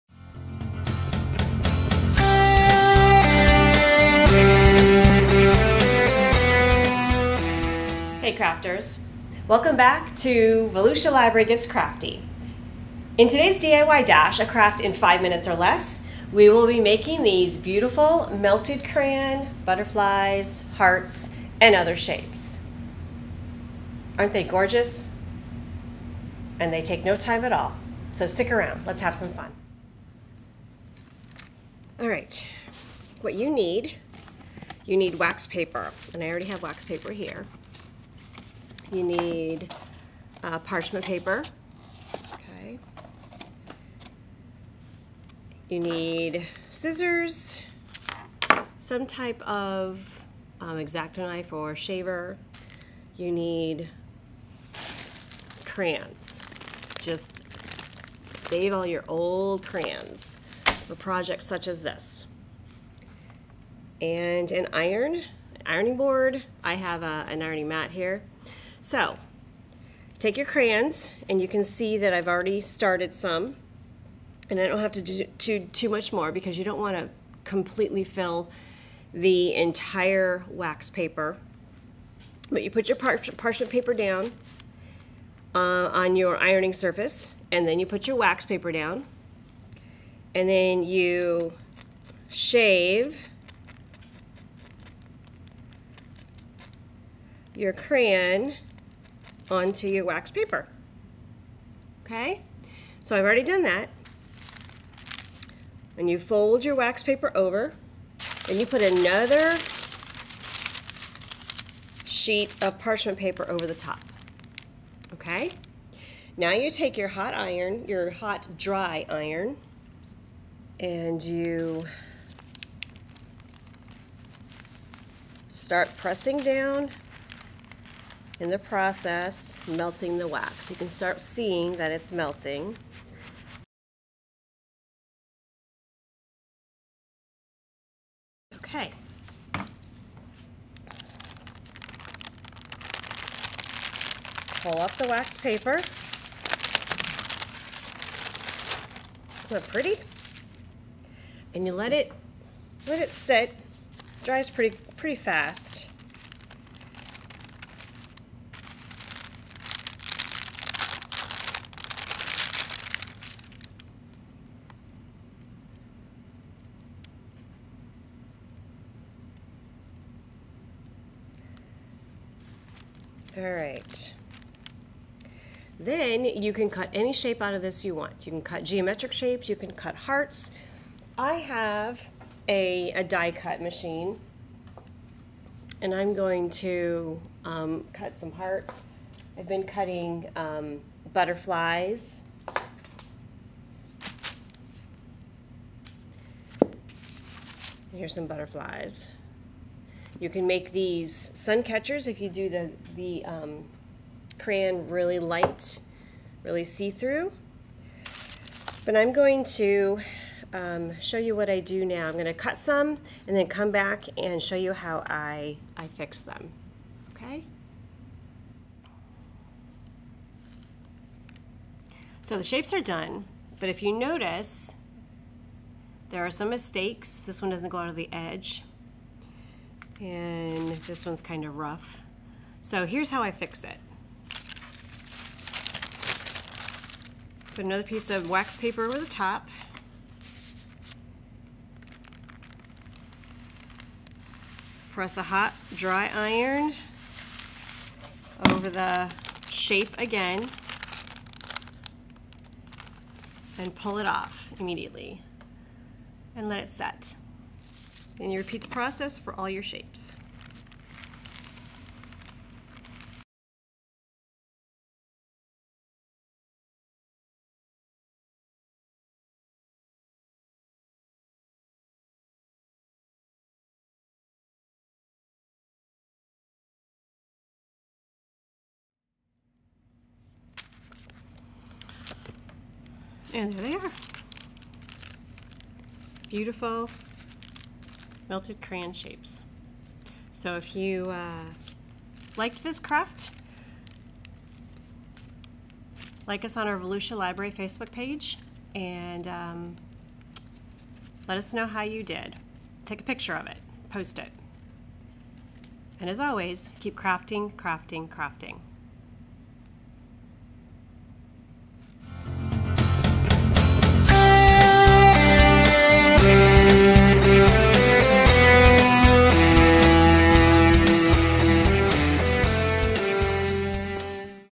Melted Crayon Butterflies
Rhoda demonstrates how to make melted crayon butterflies and hearts that can be used in any mixed media creation.